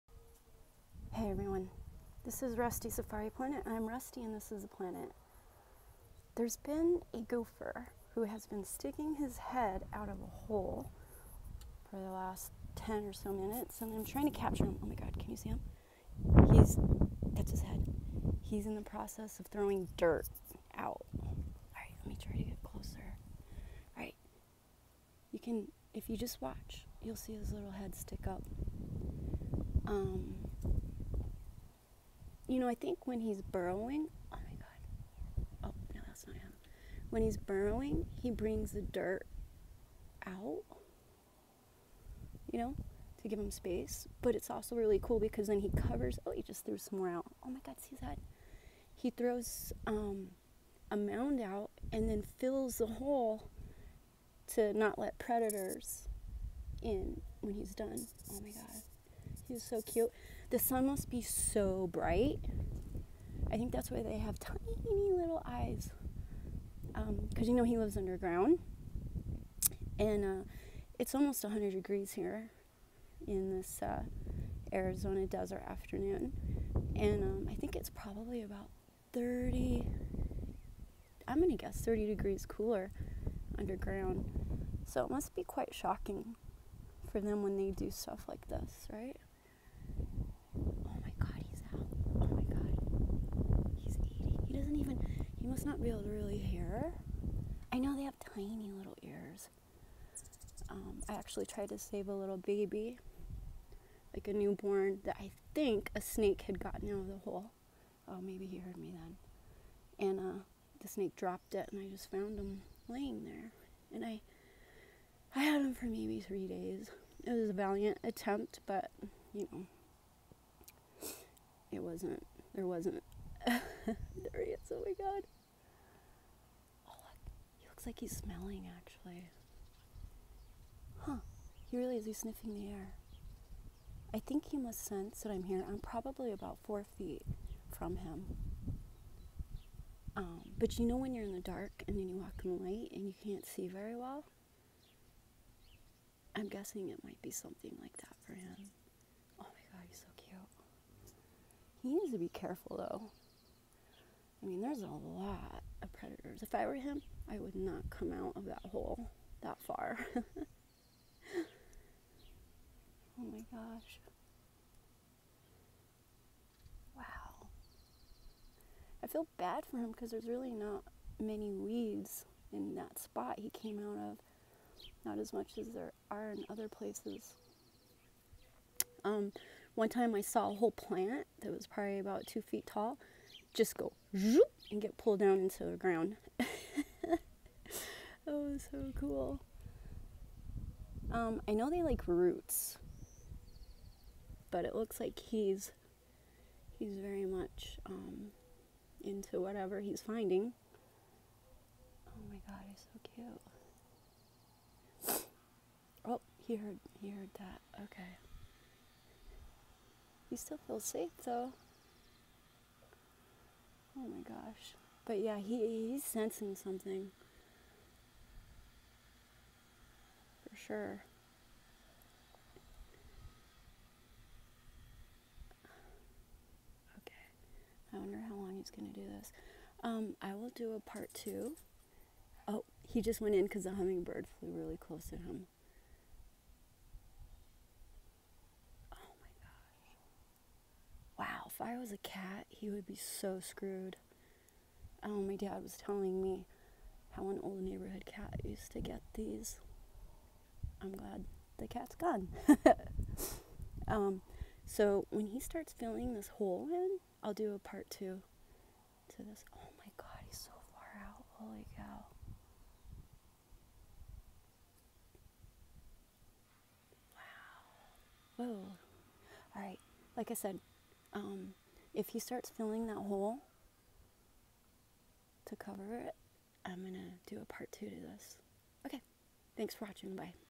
A Gopher foraging - Part 1
Hang out with a gopher collecting it’s dinner...(sorry for sloppy camera work, tsk tsk)